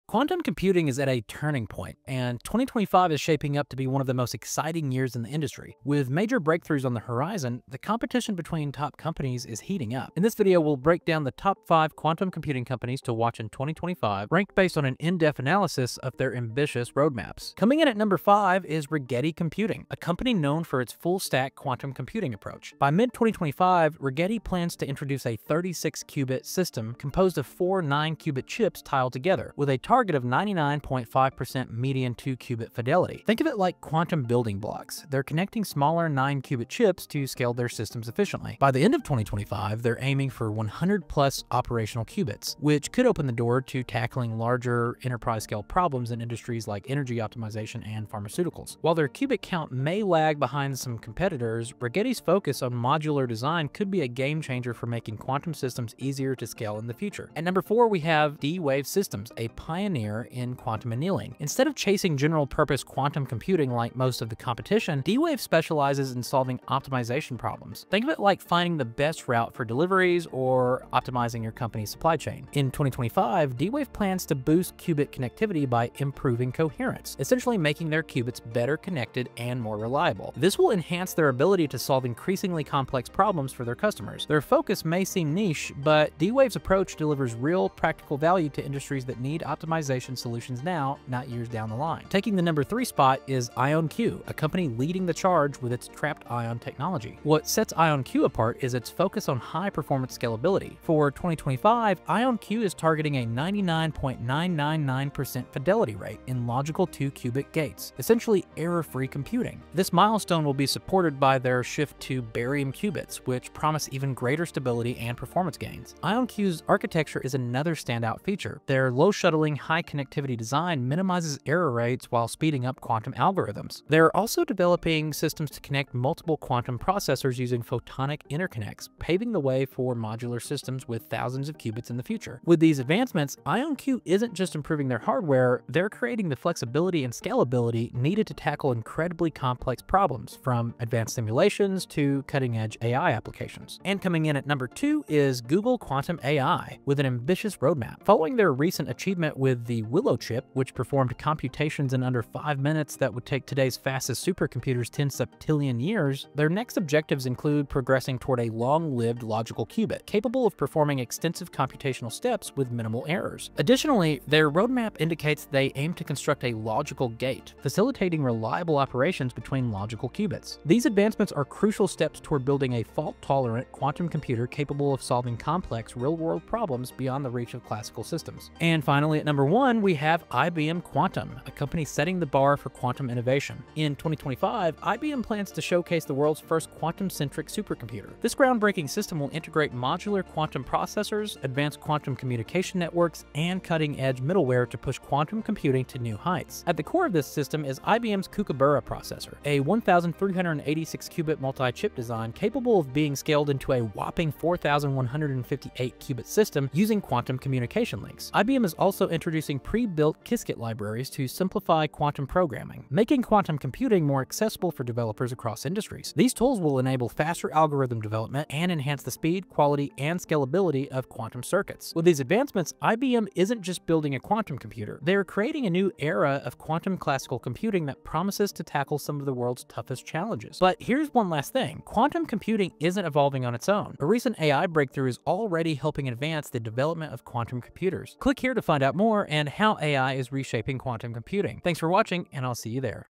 5 Quantum Computing Companies TO WATCH in 2025
Quantum computing is advancing fast! Discover the Top 5 Companies to watch in 2025 and their game-changing plans. Which one do you think will lead the industry? 🚀

Quantum computing is at the edge of a massive breakthrough in 2025. In this video, I break down the Top 5 must-watch quantum computing companies, ranked based on their ambitious roadmaps and innovations for the coming year. From modular supercomputers to near error-free qubits, these companies are paving the way for quantum technology to transform industries like AI, logistics, pharmaceuticals, and beyond.

💬 Which of these companies do you think will dominate in 2025? Share your predictions in the comments!